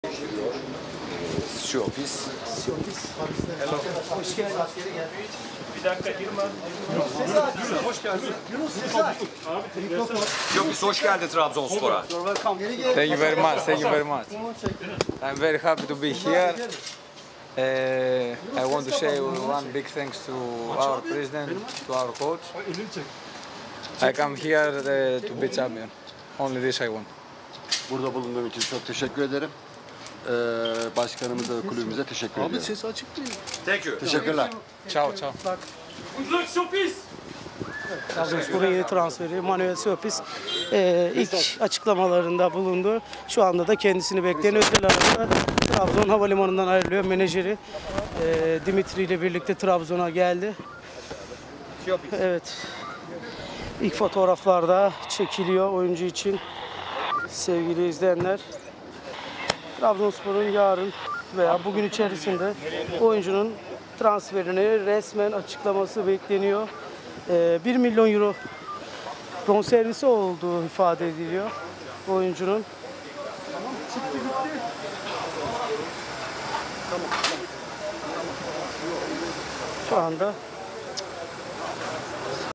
Trabzonspor, Manolis Siopis'i Trabzon getirdi
Transfer çalışmalarını sürdüren Trabzonspor, Alanyaspor'dan Manolis Siopis'i Trabzon'a getirdi.